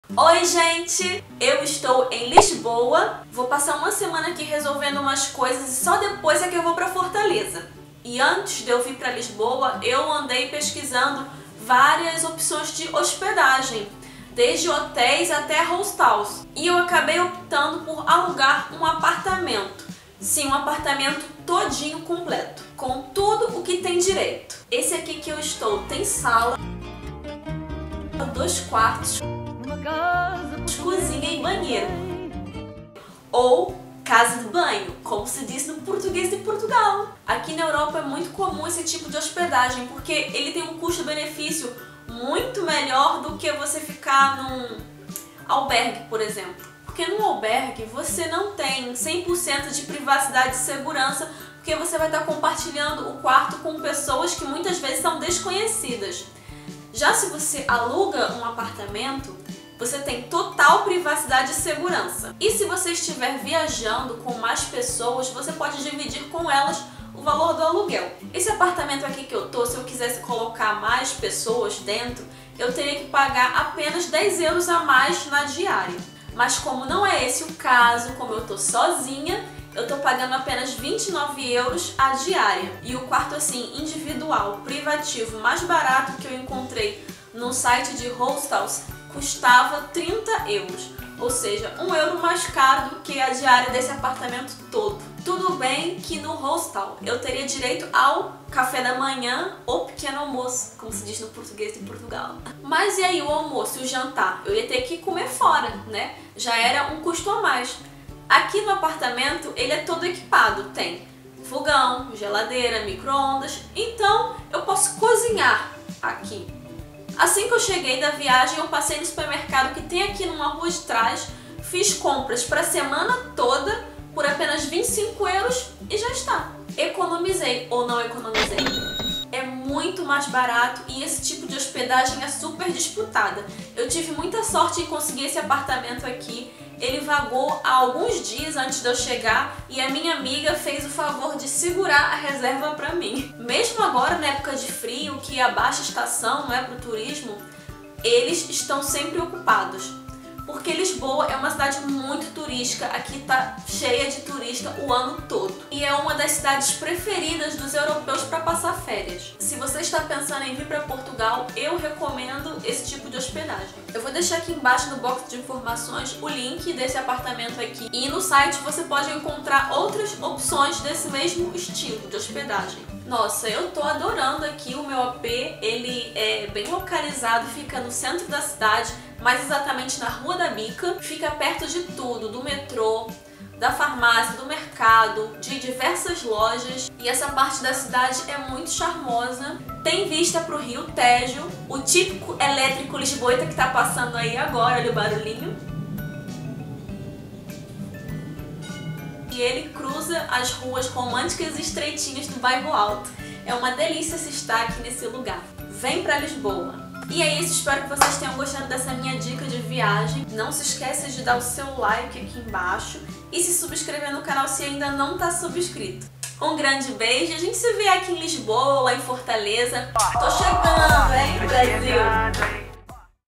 Onde Ficar em Lisboa Pagando Barato e com Conforto | Airbnb
Conheçam o apartamento onde eu fiquei em Lisboa e saibam como é fácil, barato e confortável esse tipo de hospedagem.